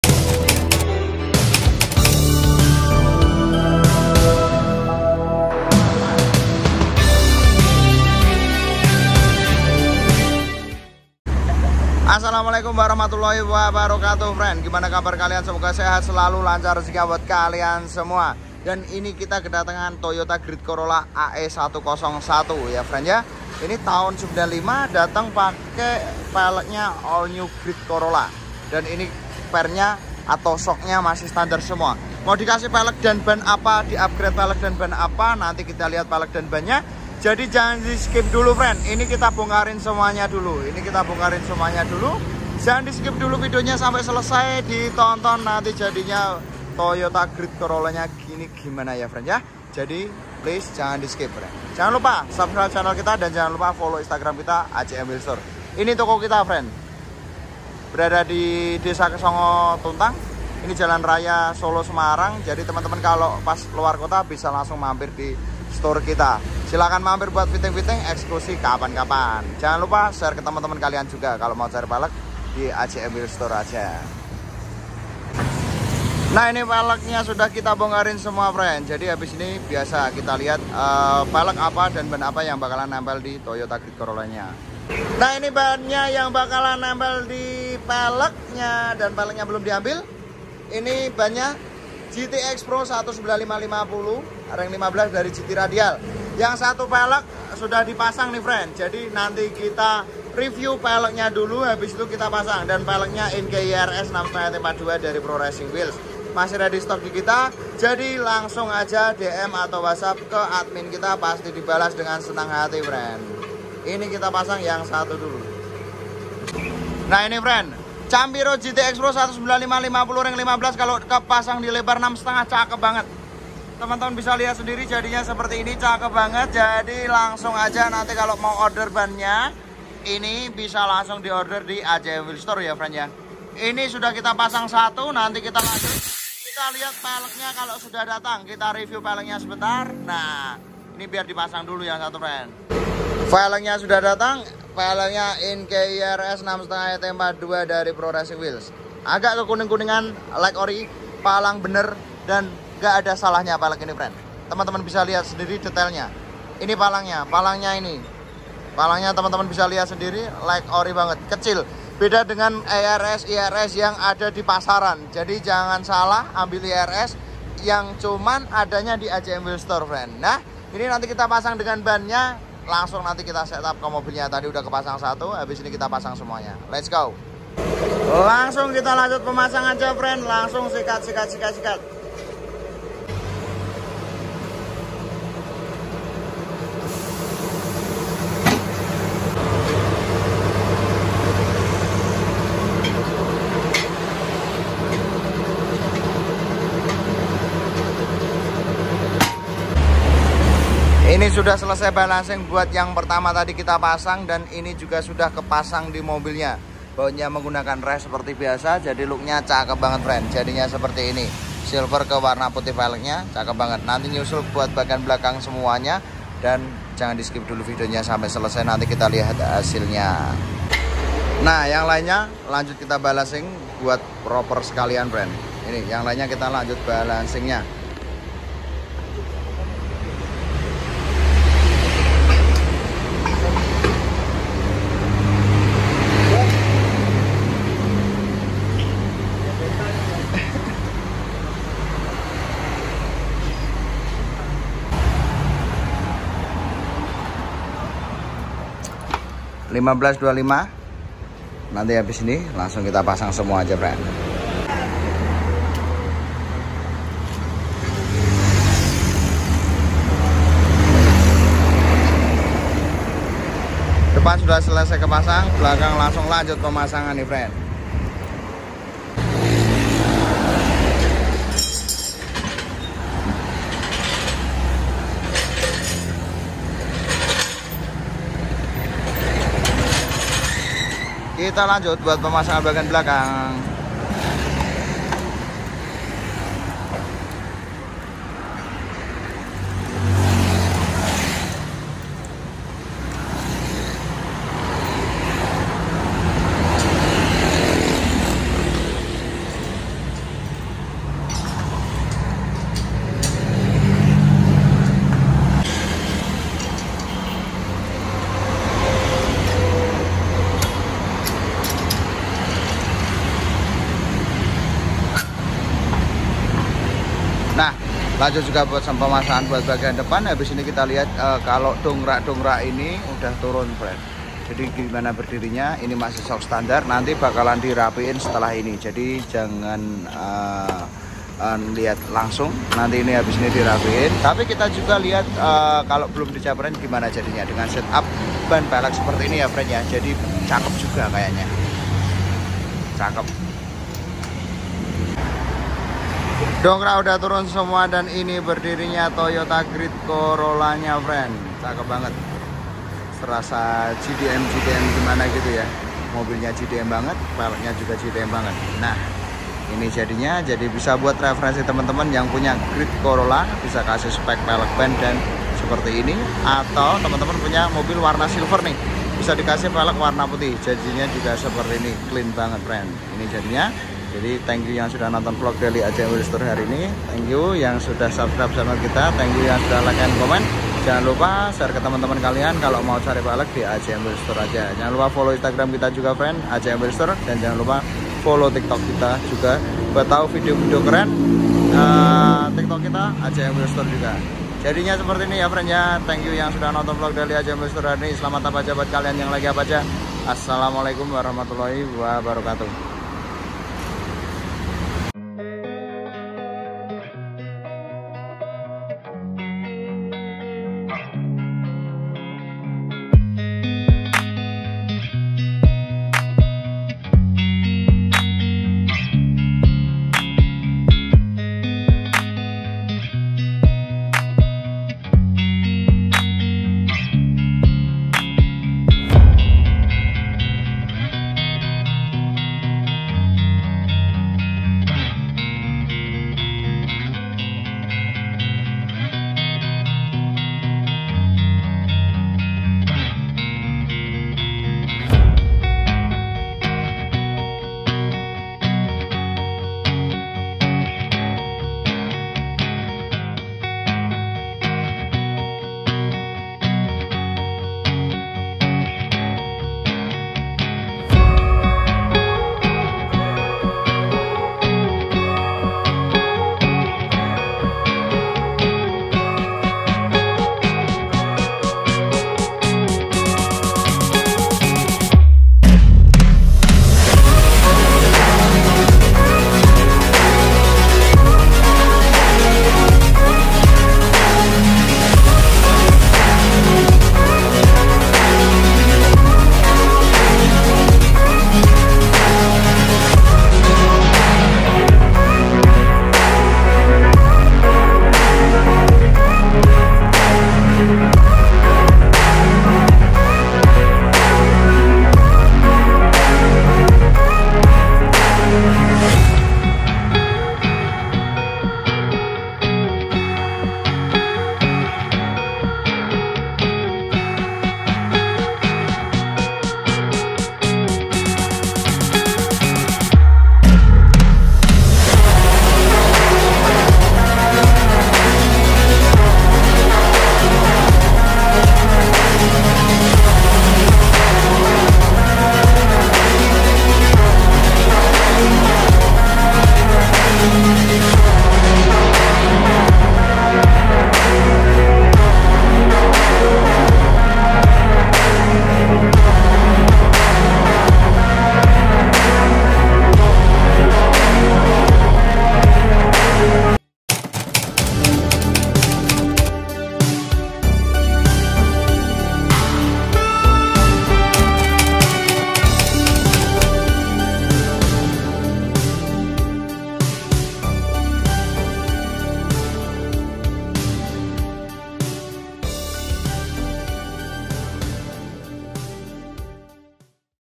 MODIFIKASI GREAT COROLLA AE101 YANG PAS DAN PROPER ❗️❗️❗️
VELG :

ENKEI IRS
RING 15
LEBAR 6,5 ET42
PCD 4X100

BAN :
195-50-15
CHAMPIRO GT-X PRO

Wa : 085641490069